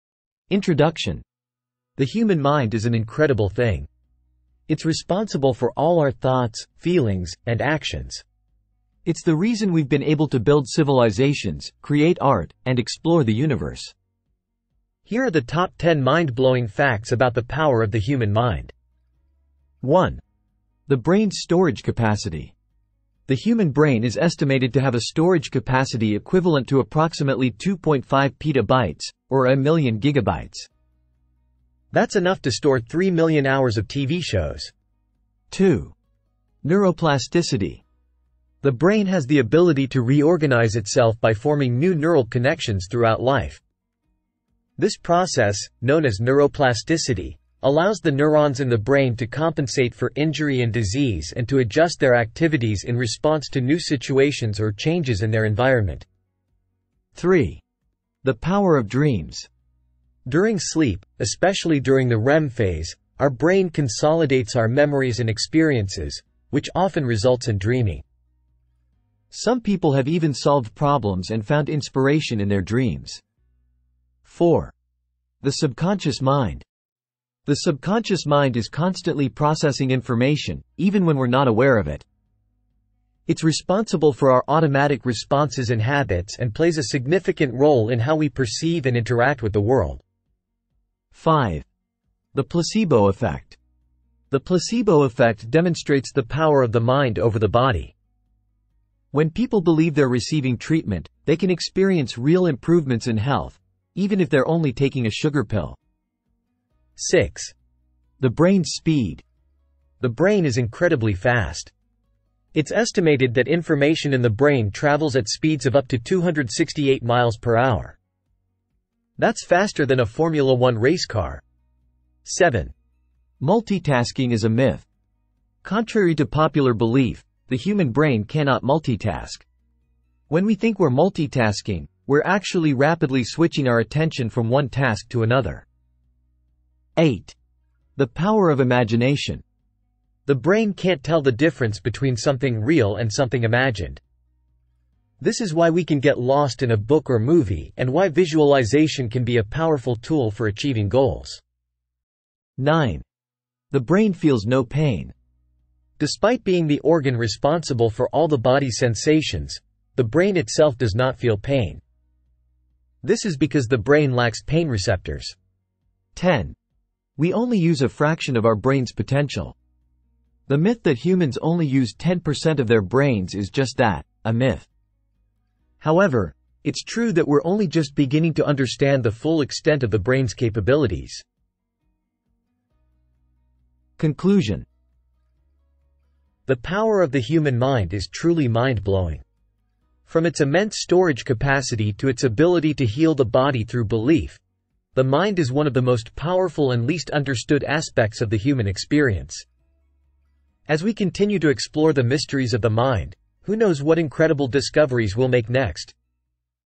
Top 10 Mind Blowing Facts About the Power of the Human Mind
Unlock the mysteries of the human mind with these top 10 mind-blowing facts that reveal the astonishing capabilities and potential of the human brain. From cognitive abilities to neurological phenomena, these facts offer profound insights into the power of human intelligence.